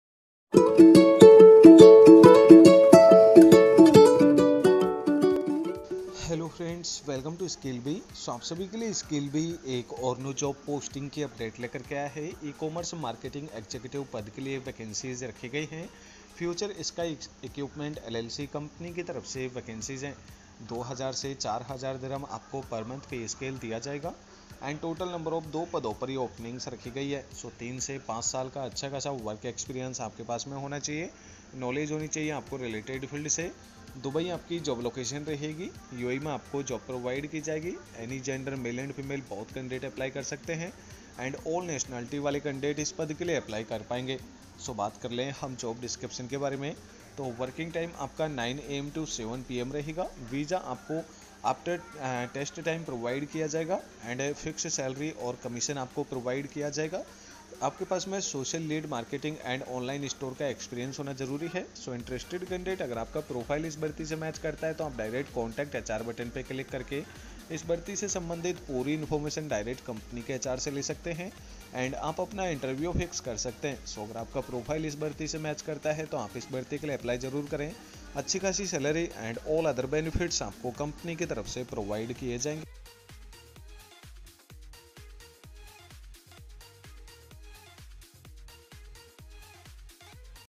JOB IN UAE / Role: Ecommerce Marketing Executive / Company: Future Sky Equipment/Salary:AED2000-4000
15. Check out this job opening on the Skillbee app that is available for you:

Role: Ecommerce Marketing Executive

Company: Future Sky Equipment Llc.

Openings: 2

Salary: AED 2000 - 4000

Experience: 3 - 5 years

Job Location: Future Sky Equipment LLC - Dubai - United Arab Emirates

Interview Location: Future Sky Equipment LLC - Dubai - United Arab Emirates

Follow SkillBee :-